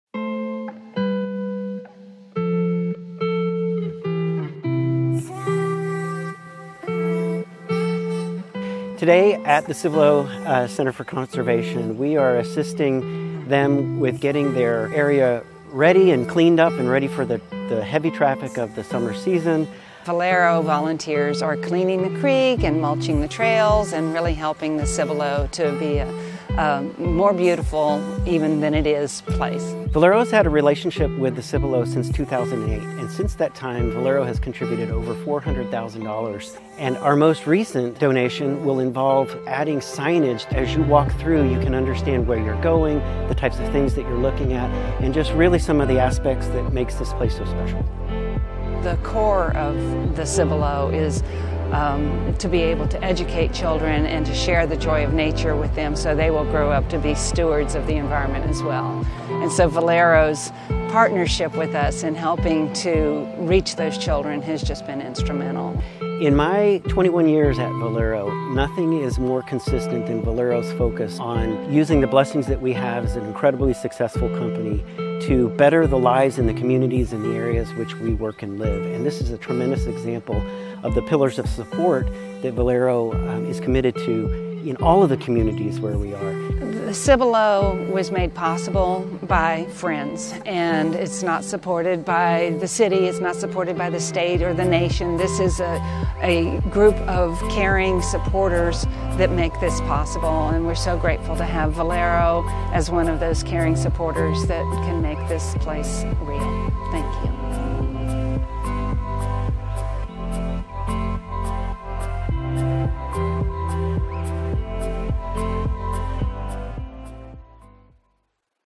Beautifying Cibolo Nature Center | Giving Back
Through the years, Valero has been a supporter of the Cibolo Center for Conservation in Boerne, Texas. Through our partnership, Valero has donated over $400,000 to support the center and its mission to promote the conservation of natural resources through education and stewardship.

Take a look at Team Valero showing how caring is in our roots, helping to beautify the nature center for the more than 100,000 children, adults, and families who visit each year.